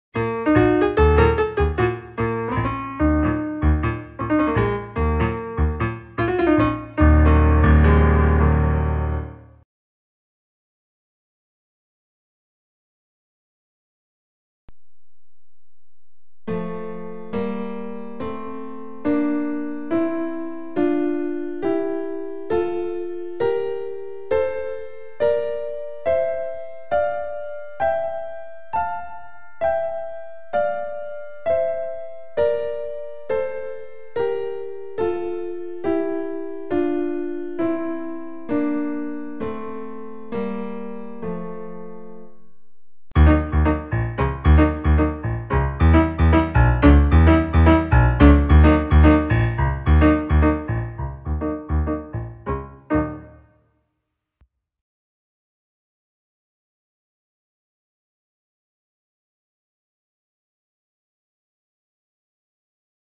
Online Piano Scales :F Major Scale 3rd Apart
Piano Lesson
Piano basics: Learn How to play piano scale F Major third Apart
Right hand starts on A key Both Hands demo